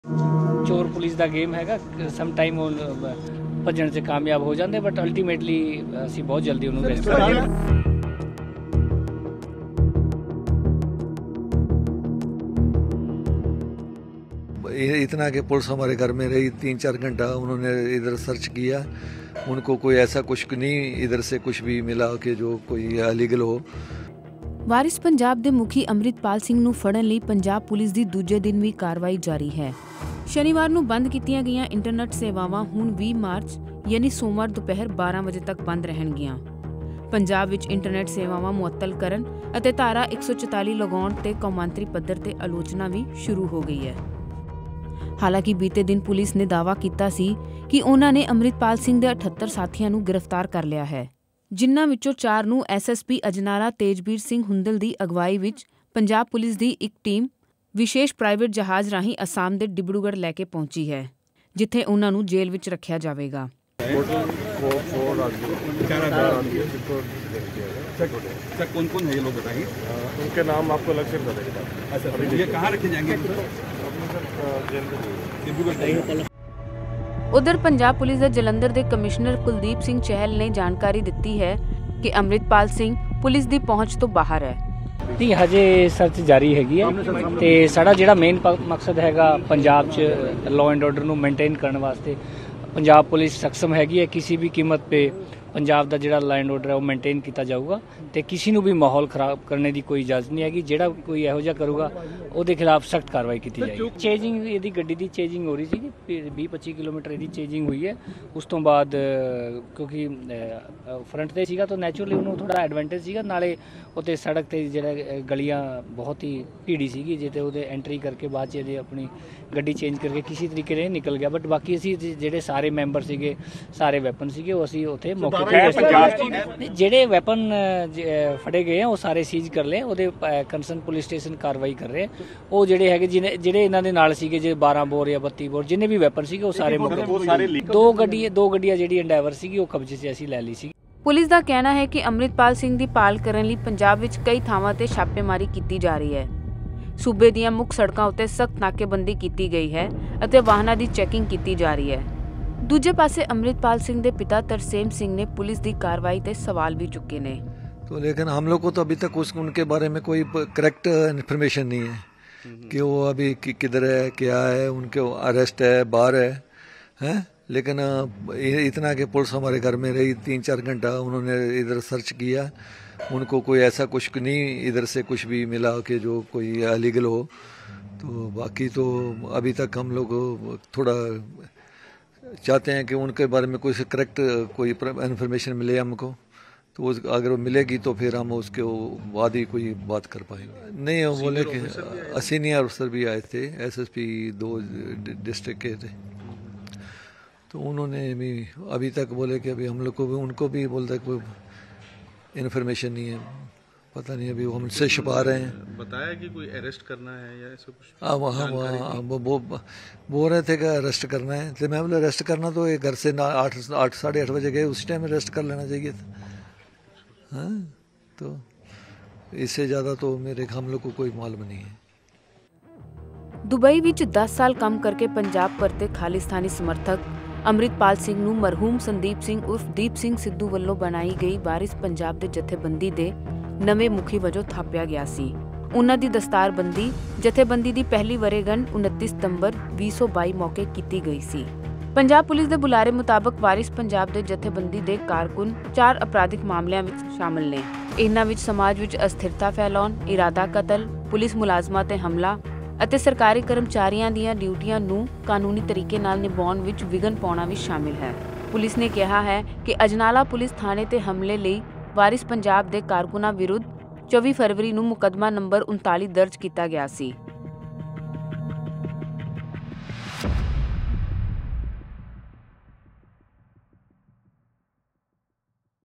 Amritpal Singh ਦੀ ਭਾਲ ਜਾਰੀ ਅਤੇ Punjab 'ਚ ਇੰਟਰਨੈੱਟ ਮੁਅੱਤਲ ਕਰਨ ਦੀ ਆਲਚੋਨਾ | 𝐁𝐁𝐂 𝐏𝐔𝐍𝐉𝐀𝐁𝐈
'ਵਾਰਿਸ ਪੰਜਾਬ ਦੇ' ਮੁਖੀ ਅਮ੍ਰਿਤਪਾਲ ਸਿੰਘ ਨੂੰ ਫੜ੍ਹਨ ਲਈ ਪੰਜਾਬ ਪੁਲਿਸ ਦੀ ਦੂਜੇ ਦਿਨ ਵੀ ਕਾਰਵਾਈ ਜਾਰੀ ਹੈ। ਹਾਲਾਂਕਿ, ਪੁਲਿਸ ਨੇ ਅਮ੍ਰਿਤਪਾਲ ਸਿੰਘ ਦੇ 78 ਸਾਥੀਆਂ ਨੂੰ ਗ੍ਰਿਫ਼ਤਾਰ ਕਰਨ ਦਾ ਦਾਅਵਾ ਕੀਤਾ ਹੈ। ਸ਼ਨੀਵਾਰ ਨੂੰ ਬੰਦ ਕੀਤੀਆਂ ਗਈਆਂ ਇੰਟਰਨੈੱਟ ਸੇਵਾਵਾਂ ਹੁਣ 20ਮਾਰਚ ਯਾਨਿ ਸੋਮਵਾਰ ਦੁਪਹਿਰ 12 ਵਜੇ ਤੱਕ ਬੰਦ ਰਹਿਣਗੀਆਂ।
ਐੱਸਐਸਪੀ ਅਜਨਾਲਾ ਤੇਜਬੀਰ ਸਿੰਘ ਹੁੰਦਲ ਦੀ ਅਗਵਾਈ ਵਿੱਚ ਪੰਜਾਬ ਪੁਲਿਸ ਦੀ ਇੱਕ ਟੀਮ ਵਾਰਿਸ ਪੰਜਾਬ ਦੇ' ਜਥੇਬੰਦੀ ਦੇ ਗ੍ਰਿਫ਼ਤਾਰ ਕੀਤੇ ਗਏ ਸਾਥੀਆਂ ਨੂੰ ਵਿਸ਼ੇਸ਼ ਪ੍ਰਾਈਵੇਟ ਜਹਾਜ਼ ਰਾਹੀਂ ਅਸਾਮ ਦੇ ਡਿਬਰੂਗੜ੍ਹ ਪਹੁੰਚਾਇਆ ਗਿਆ ਹੈ, ਜਿੱਥੇ ਉਨ੍ਹਾਂ ਨੂੰ ਜੇਲ੍ਹ ਵਿੱਚ ਰੱਖਿਆ ਜਾਵੇਗਾ
ਰਿਪੋਰਟ- ਪ੍ਰਦੀਪ ਸ਼ਰਮਾ ਅਤੇ ANI
ਐਡਿਟ- ਅਸਮਾ ਹਾਫ਼ਿਜ਼